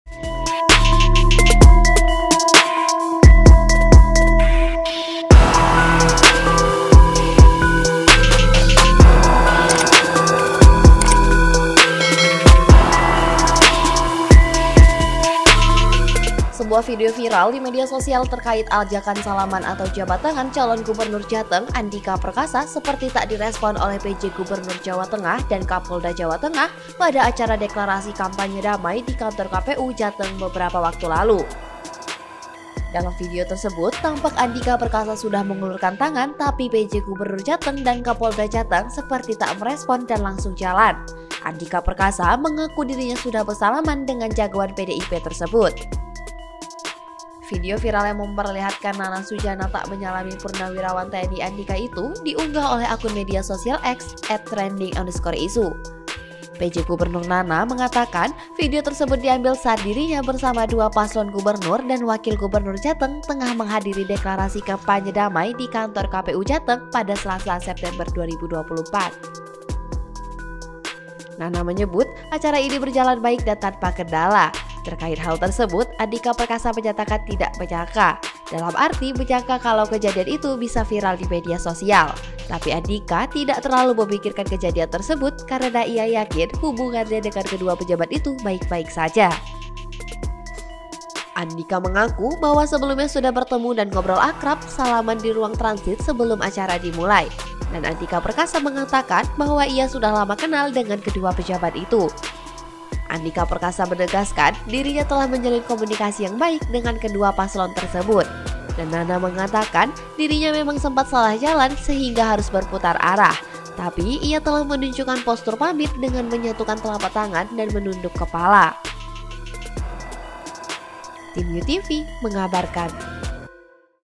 KAPOLDA HINGGA PJ GUBERNUR ABAIKAN JABAT TANGAN ANDIKA PERKASA | U-NEWS
SEMARANG - Sebuah video viral di media sosial terkait ajakan salaman atau jabat tangan calon Gubernur Jateng Andika Perkasa seperti tak di respon oleh PJ Gubernur Jawa Tengah Dan Kapolda Jawa Tengah. pada acara deklarasi kampanye damai di kantor KPU Jateng beberapa waktu lalu.

Berikut Liputannya.

Produser : Muhammad Rahmat